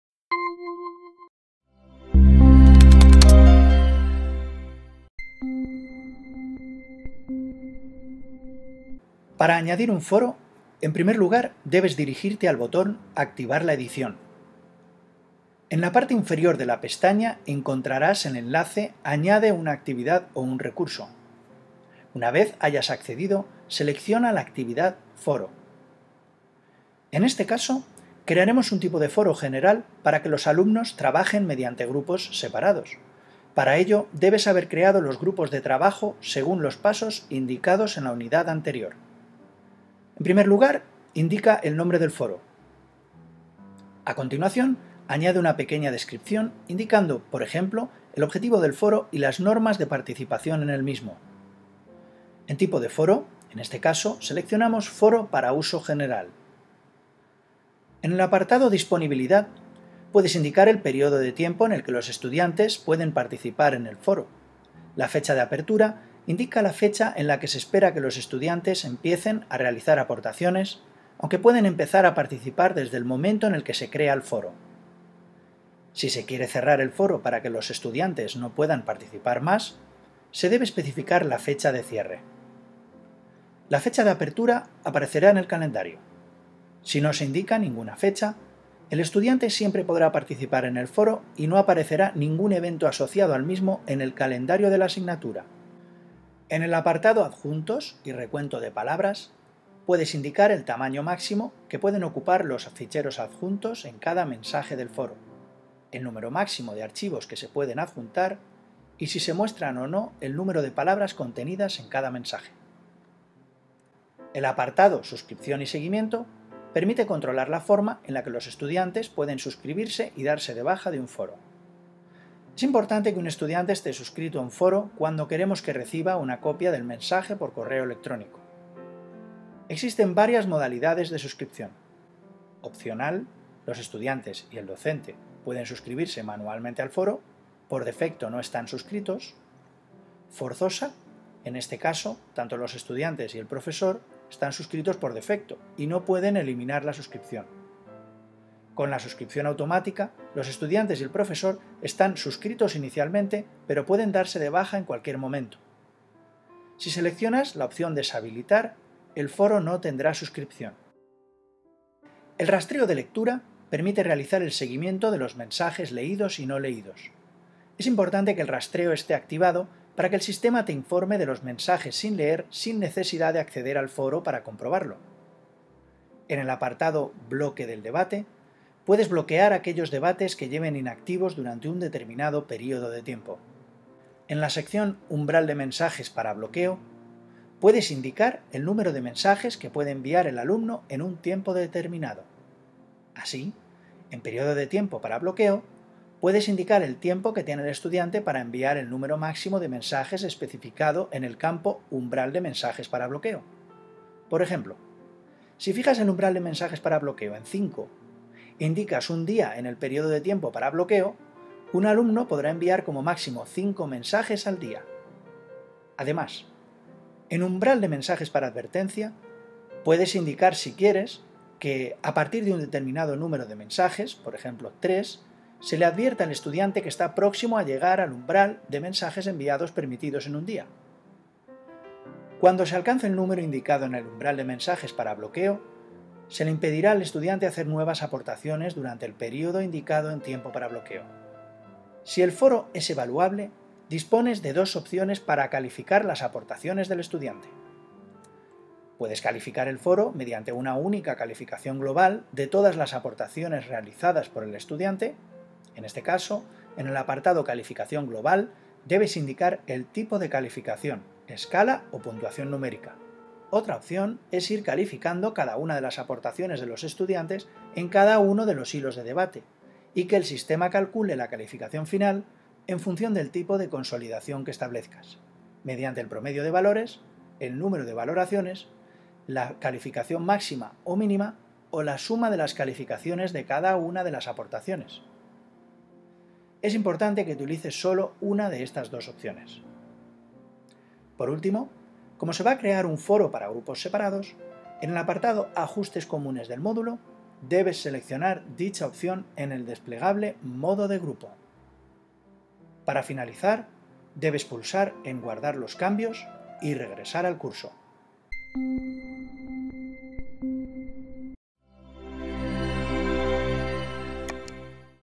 CURSO AVANZADO MOODLE 3.9 - Herramienta comunicación: Foros
Curso avanzado en el uso didáctico de Moodle 3.9 - CURSO AVANZADO MOODLE 3.9 - Herramienta comunicación: Foros

Material del Curso Avanzado en el uso didáctico de Moodle 3.9 de la Universidad Rey Juan Carlos Vídeo disponible